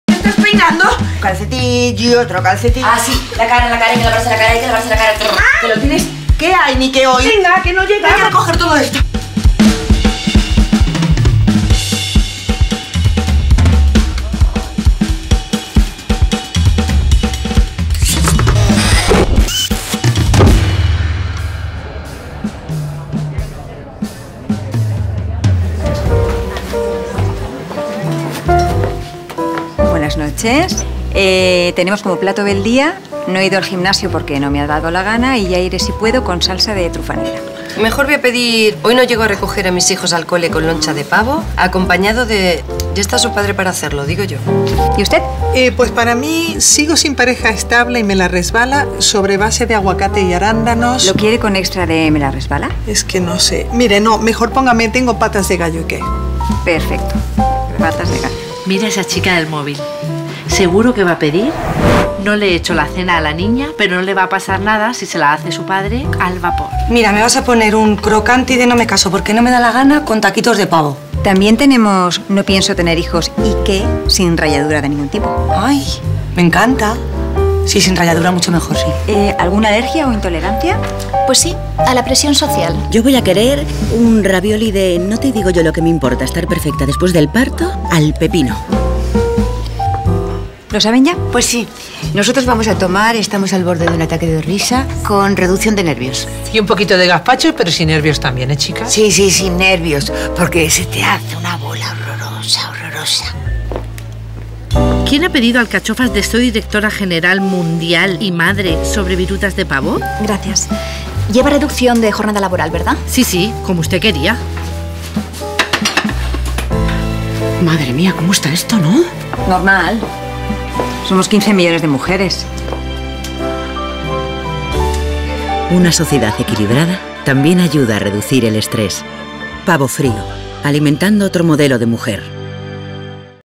Deliciosa Calma-Campofrío
Nueva campaña de Campofrío para Pavofrío, con creatividad de McCann, y protagonizada por tres chicas Almodóvar: Carmen Maura, Rossy de Palma y María Barranco.